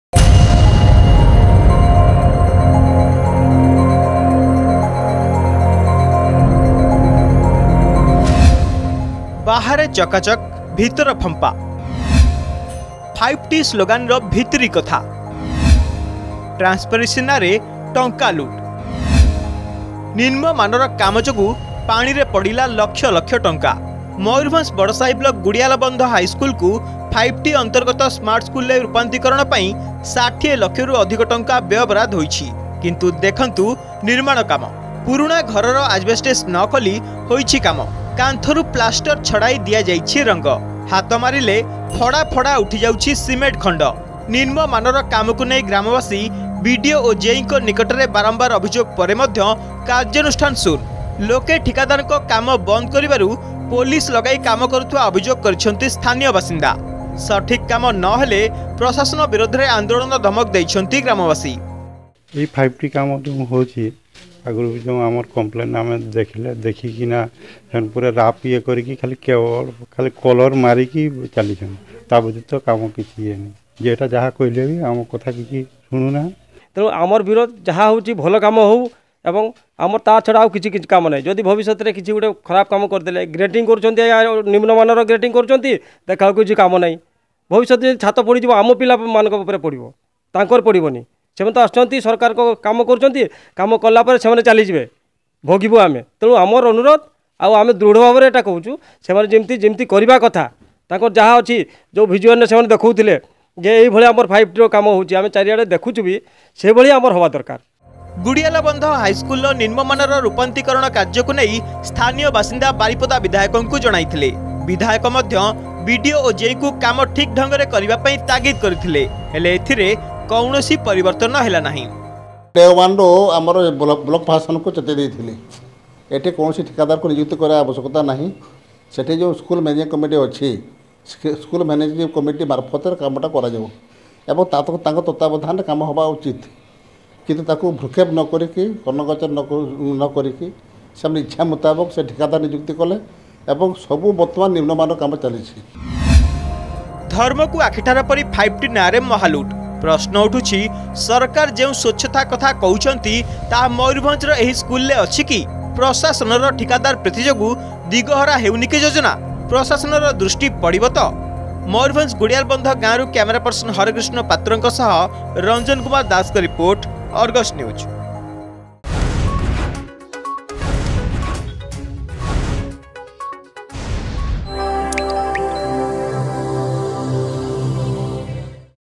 Mayurbhanj: Smart Cheating by contractor during Construction of 5T Smart Schools
Smart Cheating by contractor during Construction of 5T Smart Schools in Badasahi Village of Mayurbhanj district.